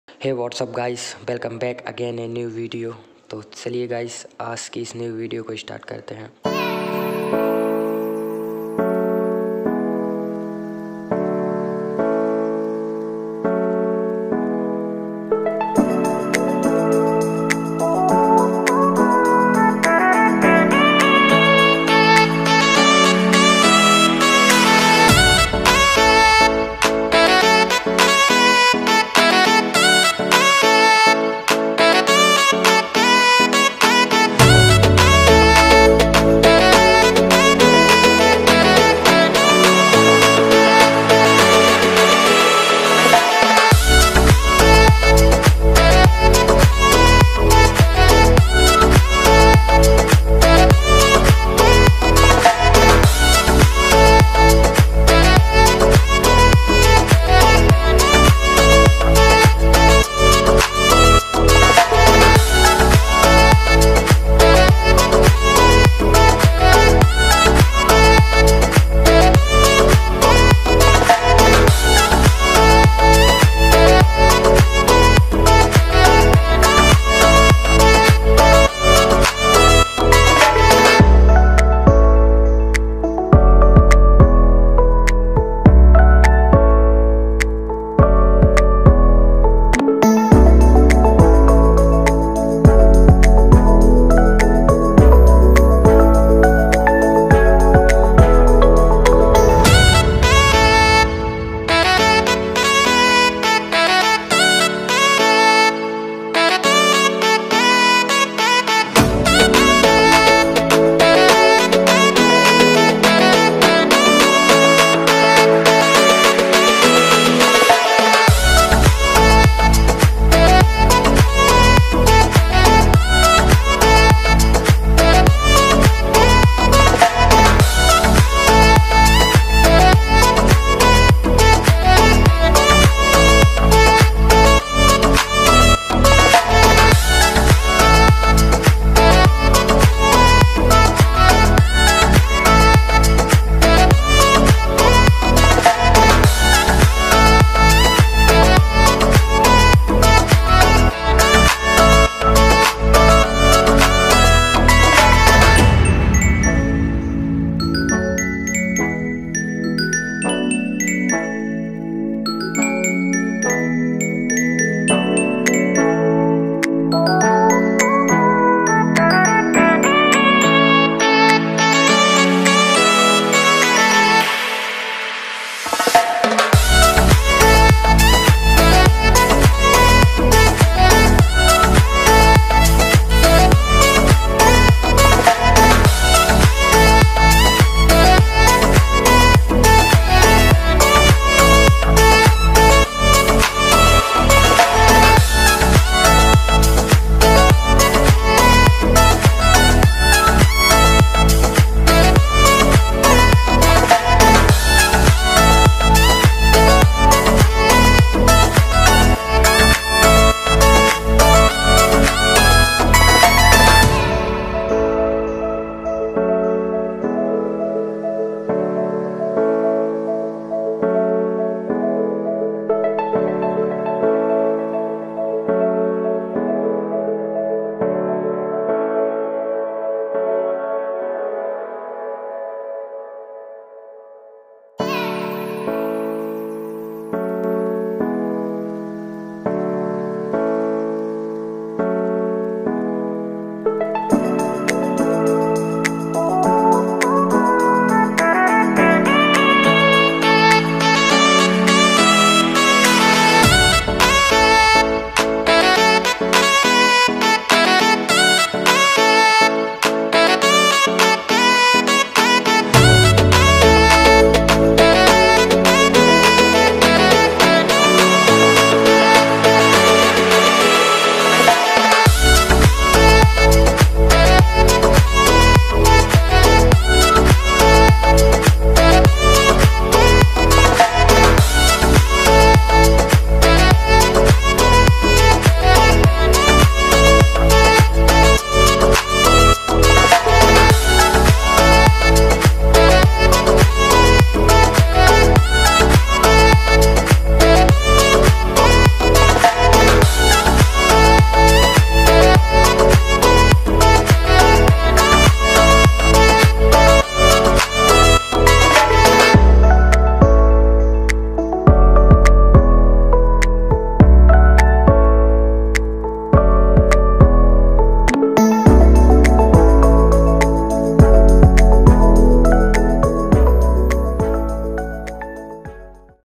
Neon Glowing Vector Art in PicsArt || PicsArt Editing || Neon Vector Art Concept || As photography
Hello beautiful people today I will teach how to make creative Ankit Singh special photo editing tutorial step by step in Hindi this video presented by All in one as photography thank you.

keyword...

PicsArt tutorial
new Editing
new tutorial

Up next

keep support 🙏🙏